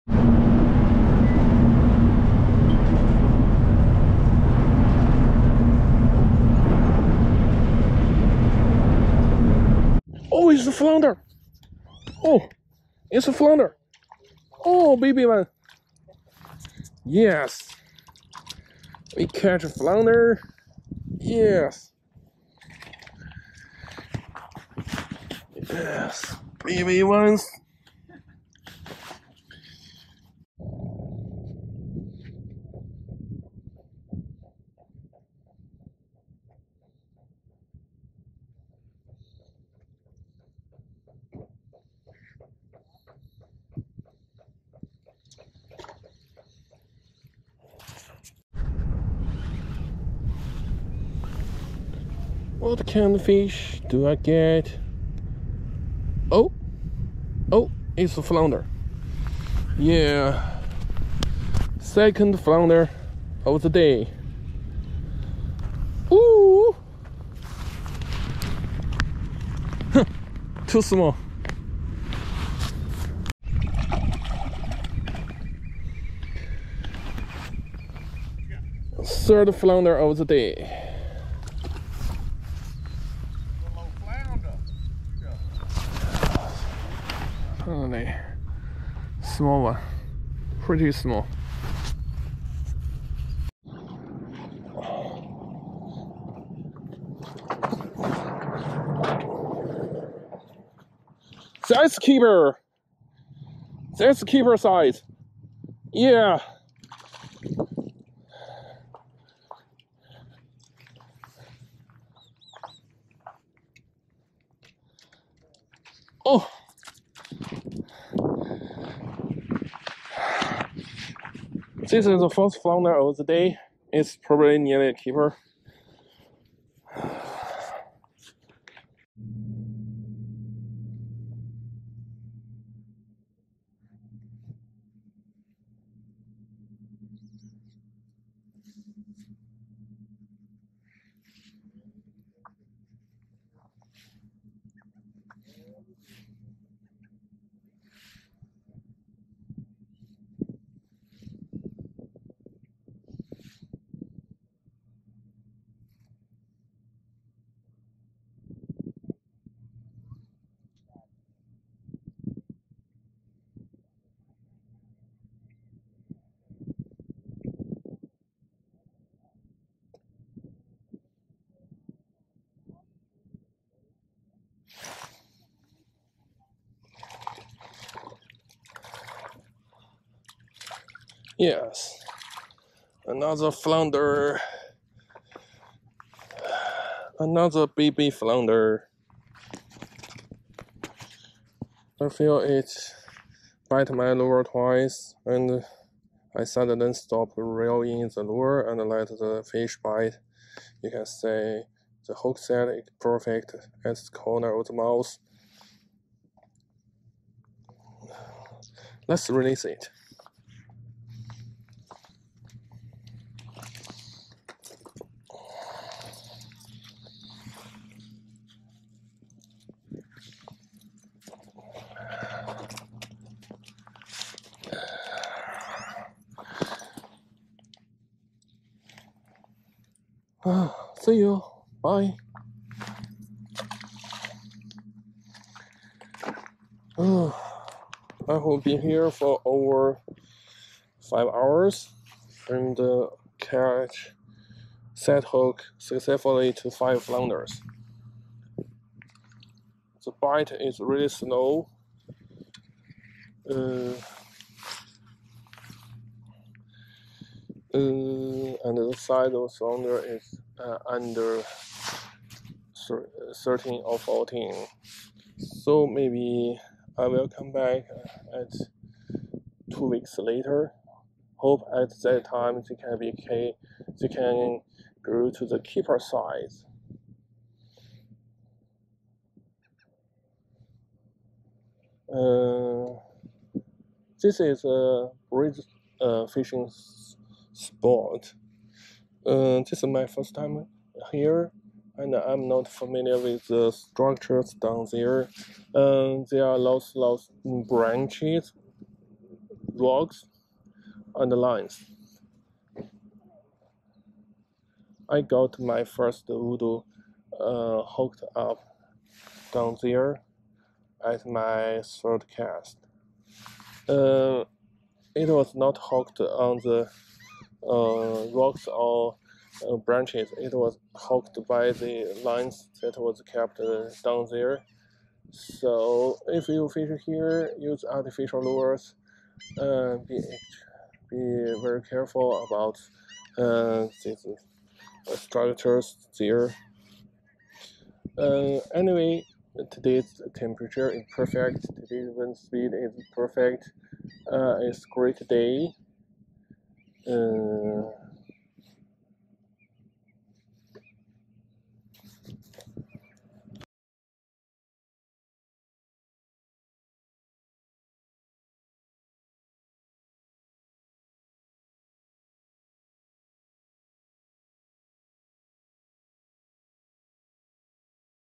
I didn't expect to catch flounders at the new spot
This is the first time that I go to the Bridge fishing spots. I was expecting to catch some trouts with small jig heads. There are no trouts today. Instead there are several baby flounders at the fishing spot. It is an amazing trip to the new fishing spot.